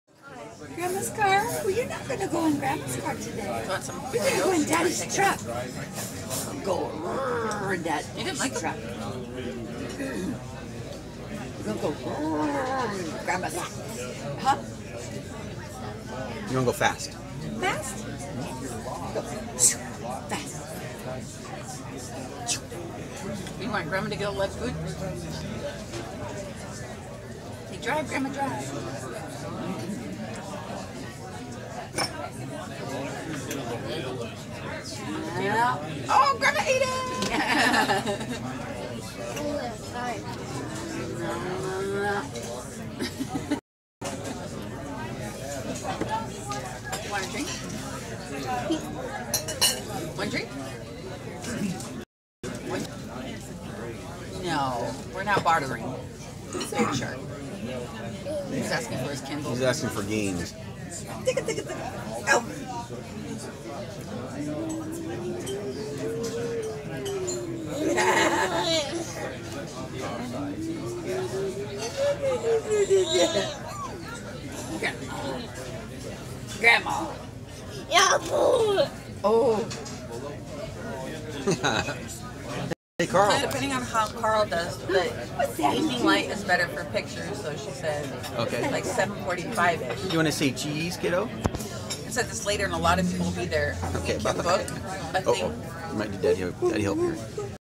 Breakfast with Grandma
Karl loves Grandma, of course!  He was really loud this morning, kind of a stinker.  Other tables were very nice, as usual.  We love the positive interaction between Karl and almost everyone we meet.